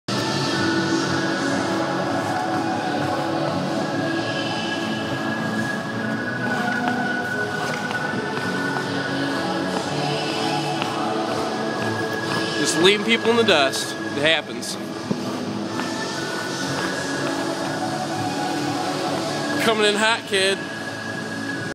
Ice Skating at Queenstown Ice Arena
Dance night in QT NZ ice arena